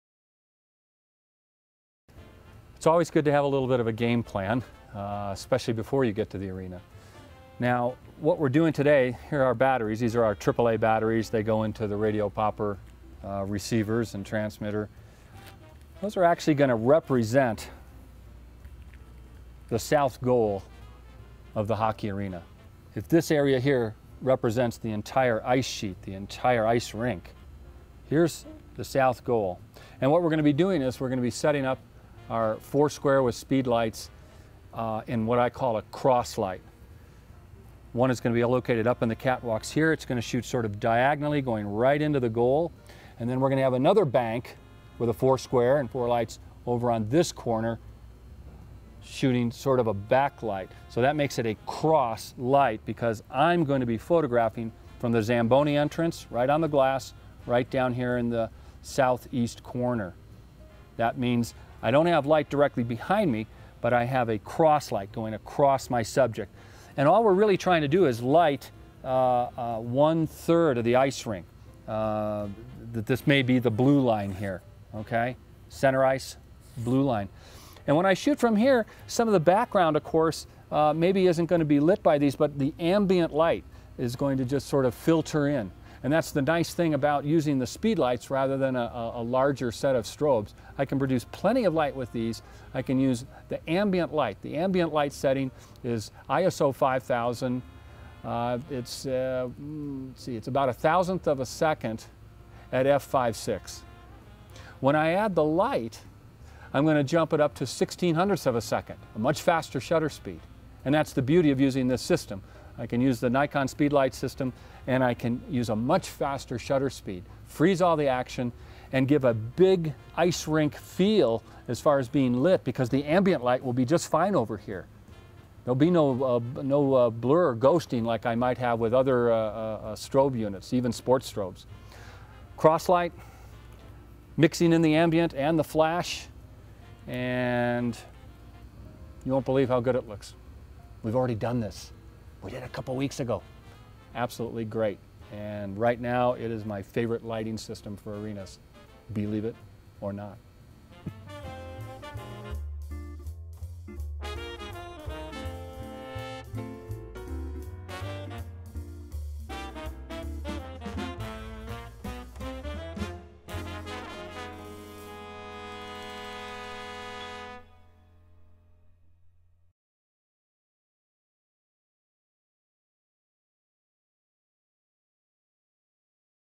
Part 3 - Dave Black Shoots Hockey with the FourSquare
in Part 3 Dave goes over the game plan for the shoot and explains where he's placing his lights.

Part 3 of a series of 5 videos with Dave Black shooting a hockey game in an indoor arena. Demonstrating everything from what he takes with him on shoots, to how he uses the FourSquare to mount speedlights up in the catwalk to illuminate the rink below as well as some video footage of him shooting and examples of the  photos he's taking.

and last but not least
Canon USA